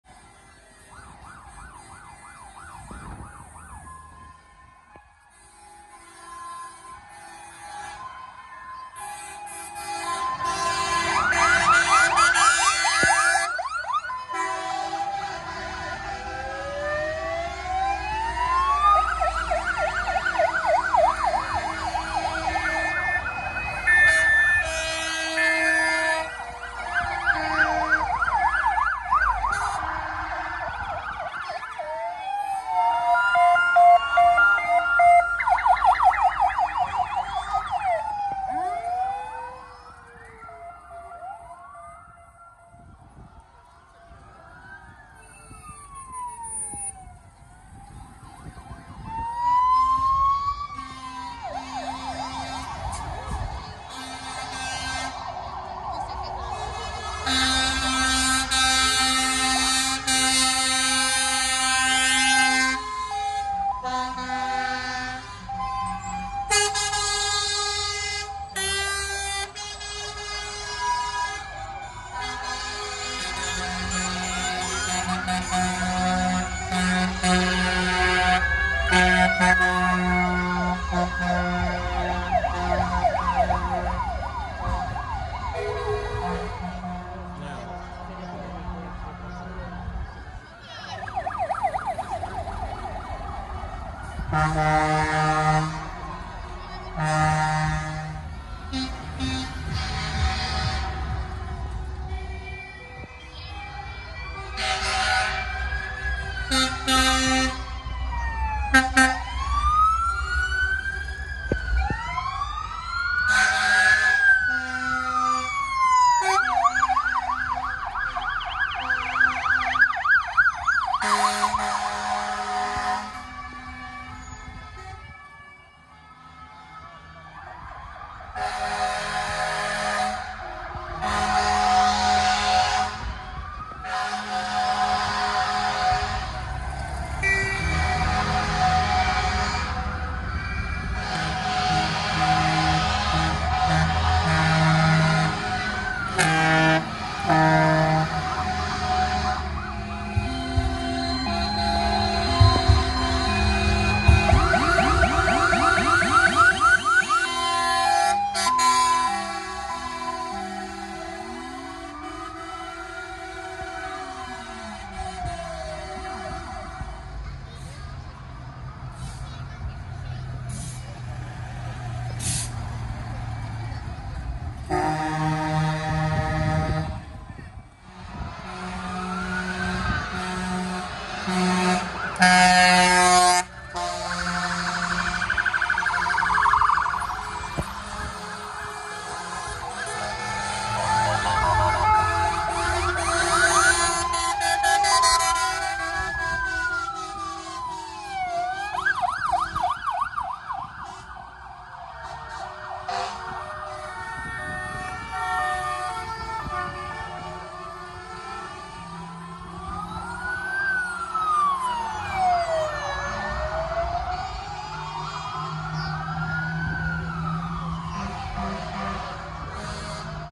Przejazd Alarmowy na VII Ogólnopolski Zlot Czerwonych Samochodów 10.08.2024
VII Ogólnopolski Zlot Czerwonych Samochodów
Było na nim 38 aut